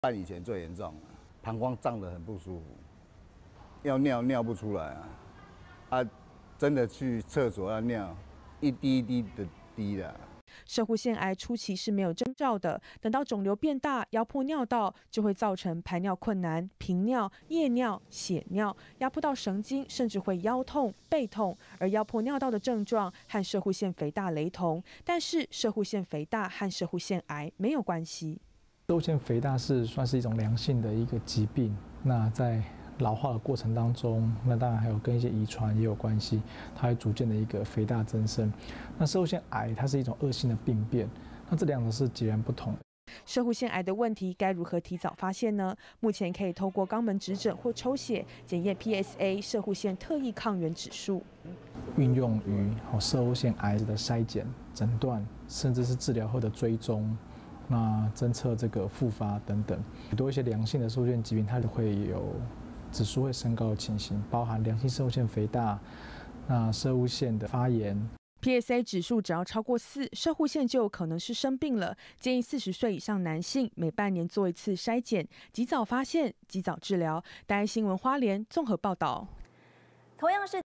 男性膀胱脹又尿不出 這裡可能有問題...
健康密碼360，今天的關鍵字是40。帶您看台灣男性十大癌症死亡之一的攝護腺癌。超過「40歲」的男人，都該注意這個隱形殺手。

攝護腺癌患者 宋先生：「一年半以前，最嚴重，膀胱脹得很不舒服，要尿尿不出來，真的去廁所要尿，一滴一滴地滴。」

攝護腺癌CGIN初期是沒有徵兆的，等到腫瘤變大，壓迫尿道，就會造成排尿困難、頻尿、夜尿、血尿，壓迫到神經甚至會有腰痛、背痛

而壓迫尿道的症狀，和攝護腺肥大雷同，但是，攝護腺肥大跟攝護腺癌沒有關係。

花蓮慈濟醫學中心泌尿腫瘤科主任 江元宏：「攝護腺肥大算是一種良性的疾病，在老化的過程中，那當然跟一些遺傳也有關係，它會逐漸地肥大增生，攝護腺癌它是一種惡性病變，這兩個是截然不同的。」

攝護腺的問題，該如何提早發現呢？目前可以透過肛門指診或抽血，檢驗PSA攝護腺特異抗原指數。

花蓮慈濟醫學中心泌尿腫瘤科主任 江元宏：「(PSA指數)運用於，攝護腺癌的篩檢，診斷，甚至是治療後的追蹤，偵測復發等等，很多一些良性攝護腺癌的疾病，它也會有指數會升高的情形，包含良性攝護腺肥大，攝護腺的發炎。」

PSA指數只要超過4，攝護腺就有可能是生病了，建議40歲以上男性每半年做一次篩檢，及早發現、及早治療。

花蓮綜合報導